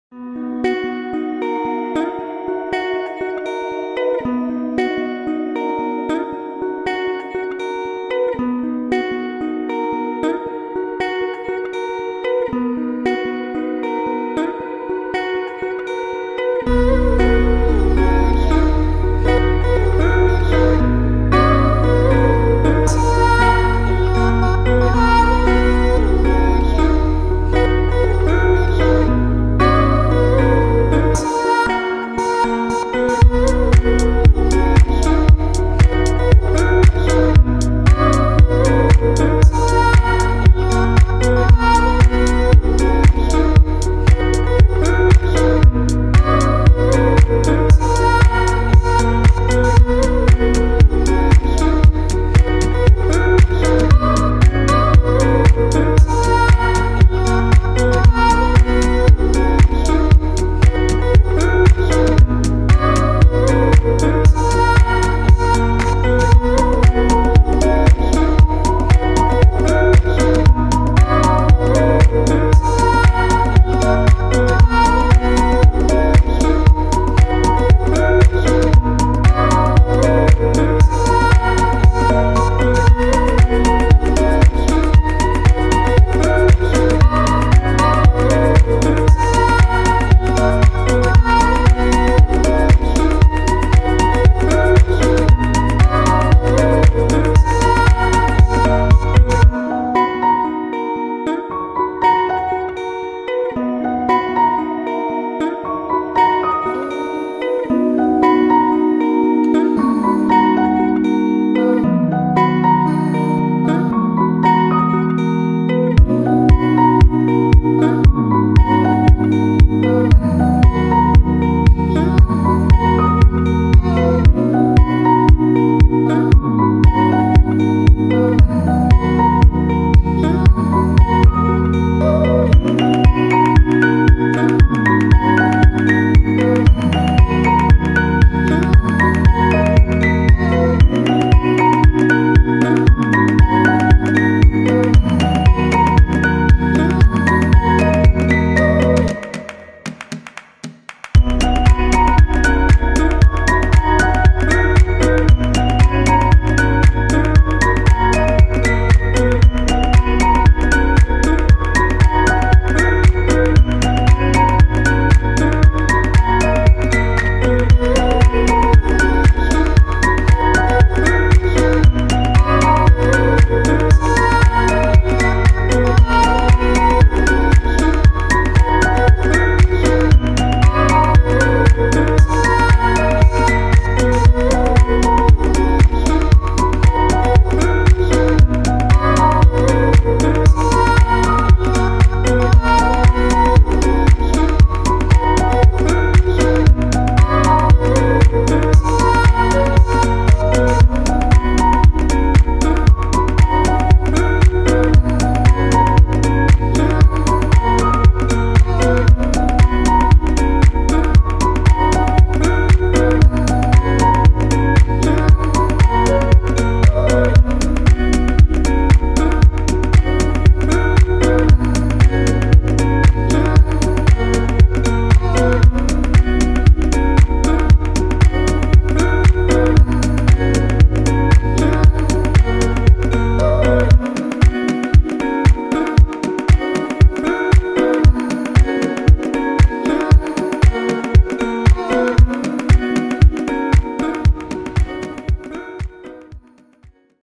Trail Forêt de Compiègne, sortie de 04h00
Préparation à l Ecotrail Paris 45km et les pyramides noires 55 km